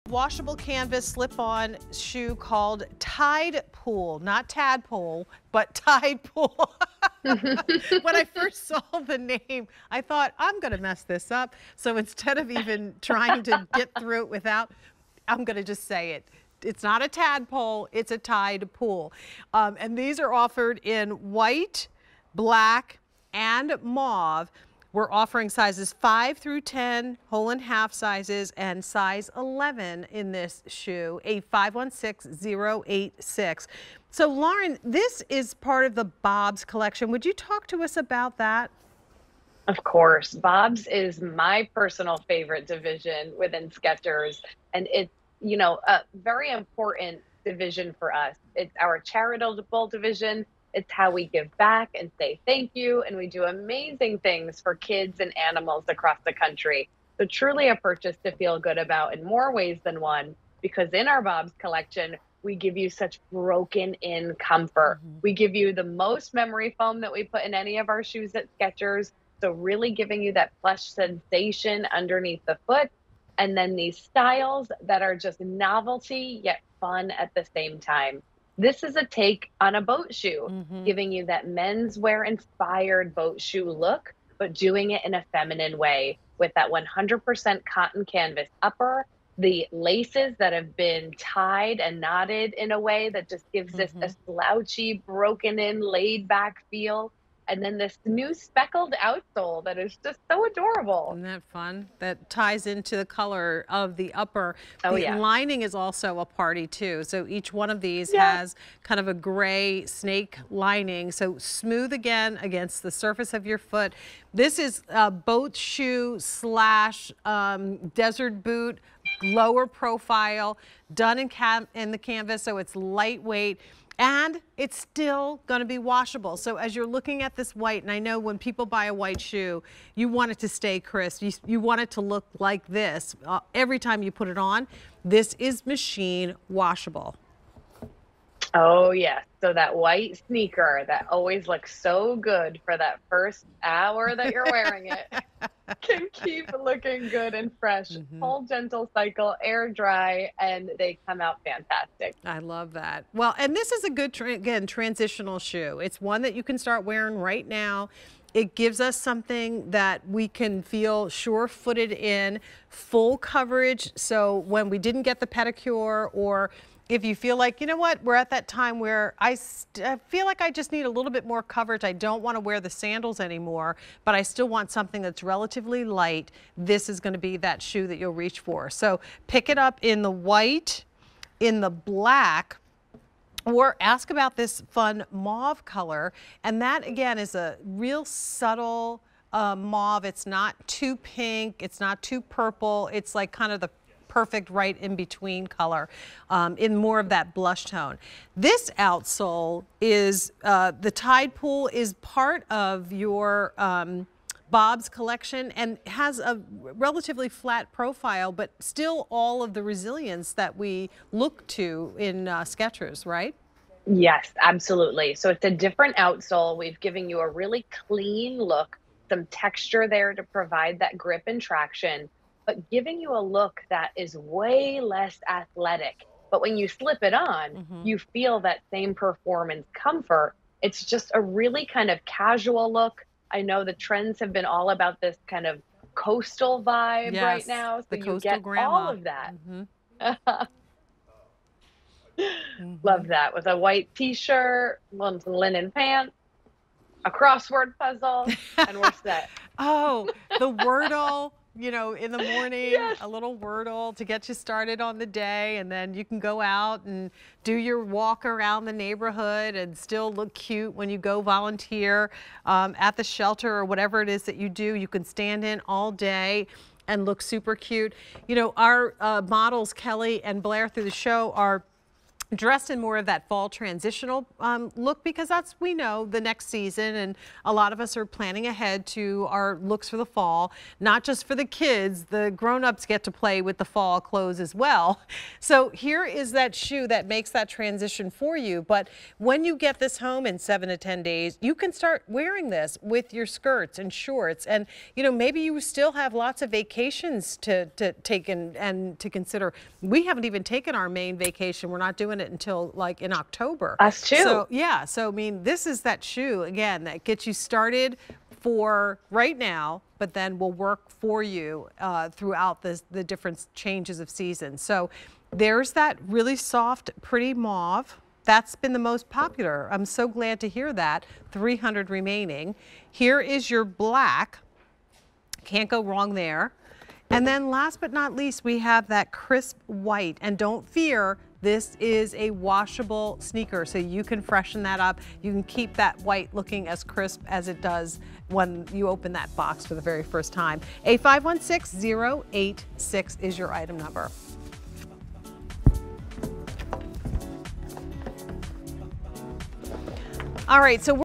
Skechers BOBS Washable Canvas Slip-Ons - Tide Pool on QVC
Skechers BOBS Washable Canvas Slip-Ons - Tide Pool

Cruise around town in these easy-wear, casually cool slip-on shoes. The cushioned memory foam insole provides all-day comfort. From Skechers.